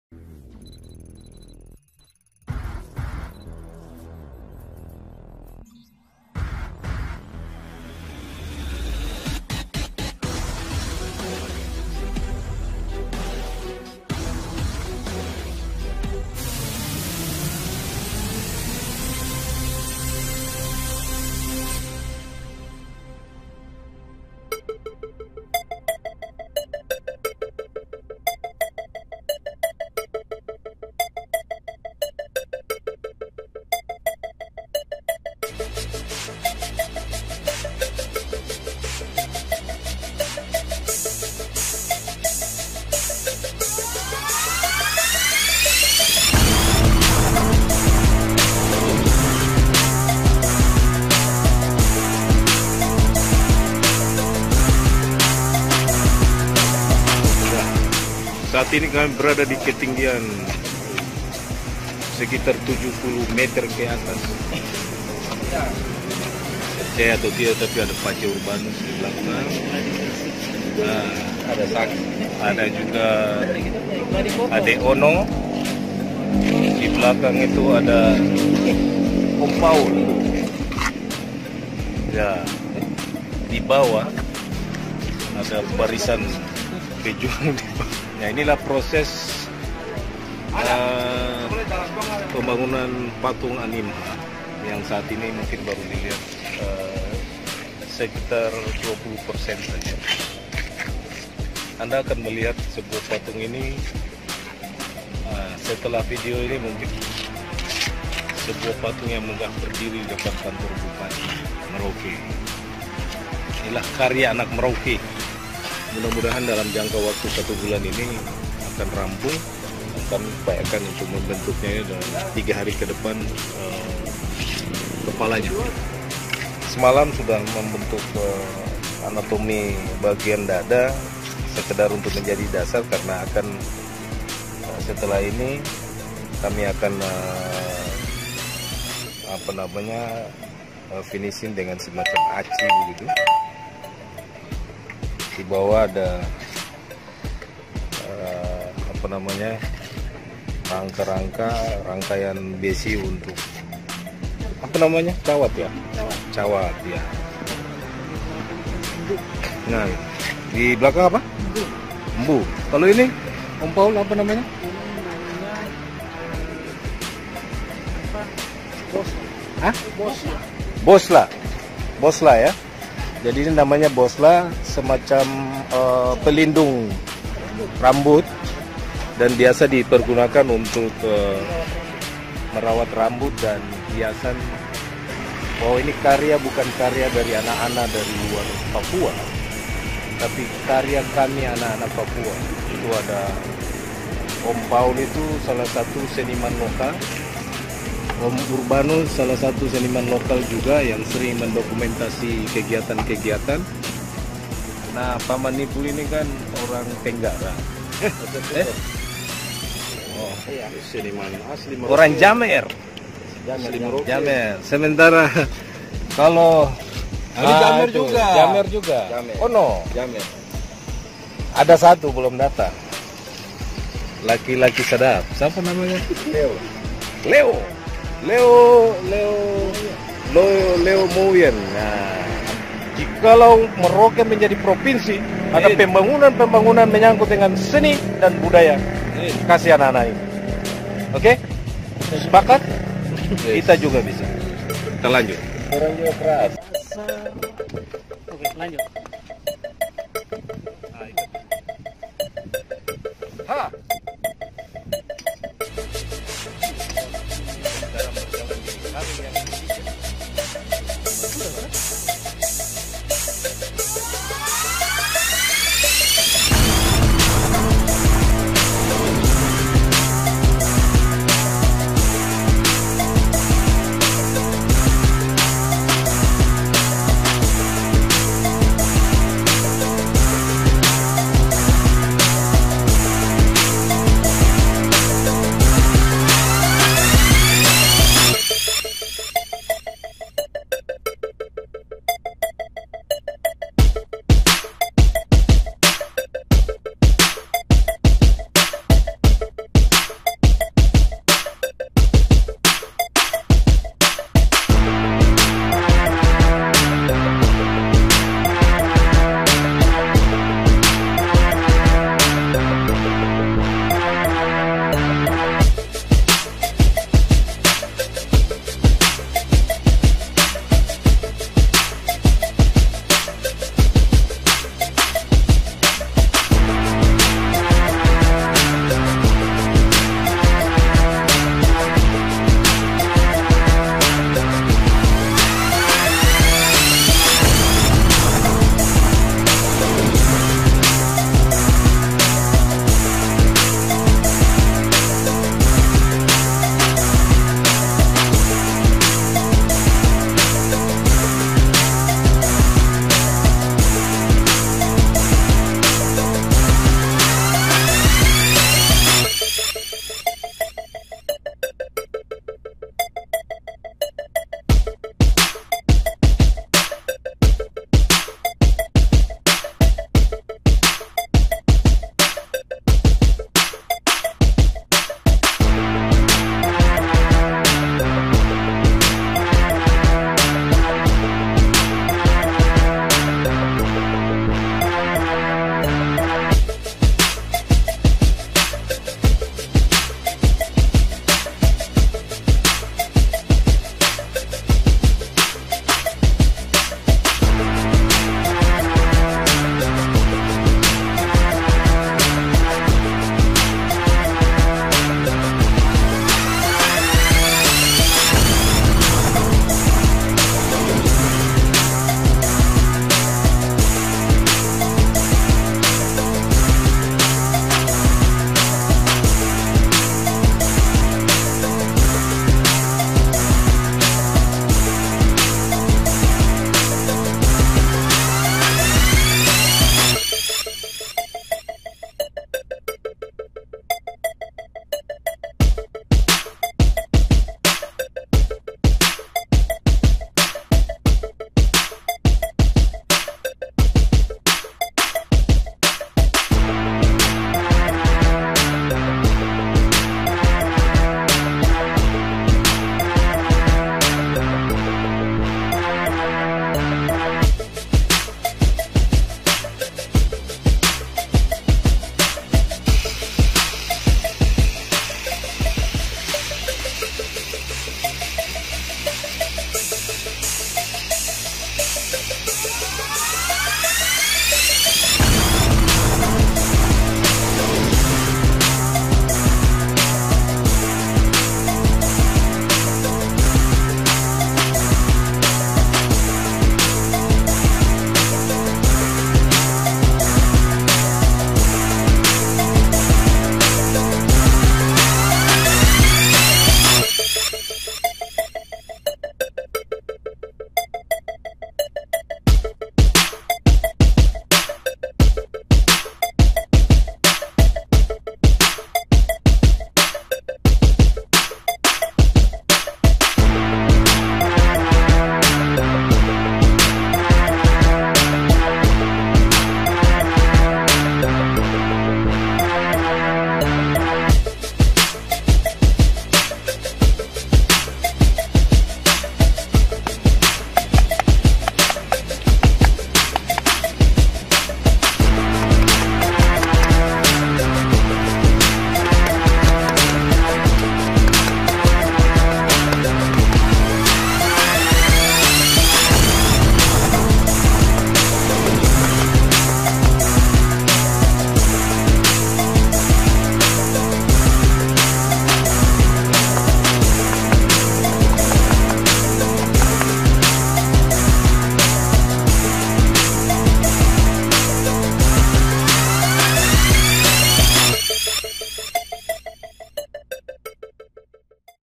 Frederikus Gebze, Kerja Dengan Kasih dan Hati : Proses Pembangunan Patung AnimHA 1
Bupati Frederikus Gebze Menjelaskan arti Animha, adalah  yang berarti manusia sejati.
"Artinya, manusia yang berguna dan berjasa. Kalau zaman dahulu, arti manusia sejati dikaitkan dengan tokoh yang mampu merebut, menguasai wilayah dan masyarakatnya," ujarnya.
Namun, menurut Freddy, seiring berkembangnya zaman, maka yang disebut manusia sejati sekarang adalah orang yang mempunyai tanggung jawab secara moril, termotivasi untuk membuat karya karya yang berguna untuk masyarakat khususnya di wilayah Merauke.
"Kalau sekarang Animha atau manusia sejati kita artikan sebagai orang yang bertanggung jawab secara moril untuk berkarya di segala bidang baik itu pendidikan, kesehatan, pemerintahan atau kerohanian," pungkasnya.